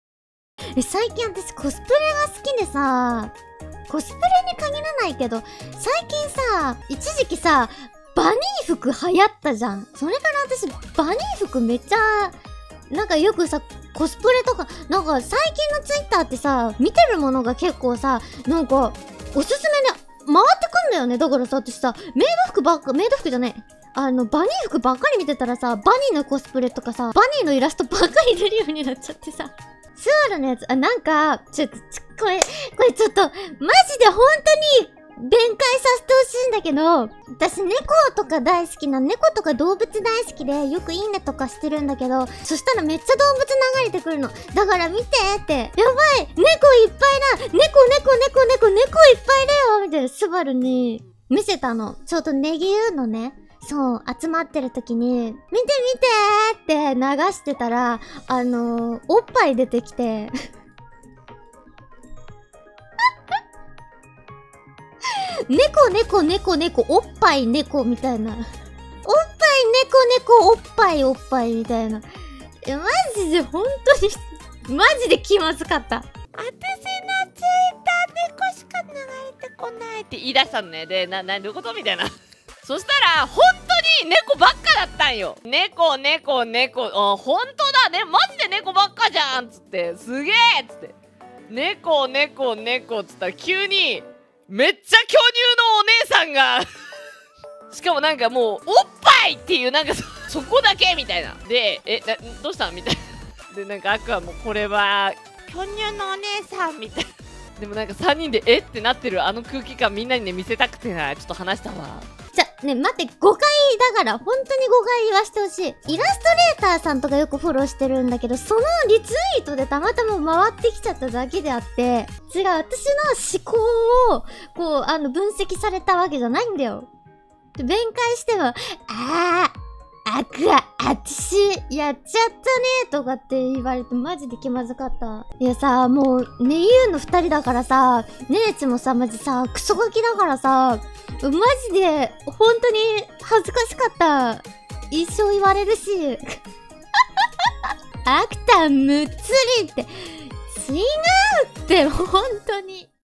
あくあのスマホのタイムラインから急にぺぇが流れてきた事件を釈明する湊あくあ【ホロライブ切り抜き/2022.05.29】
バニーはいいでしょ。

使用させていただいている素材様（敬称略）
8:51:22 pm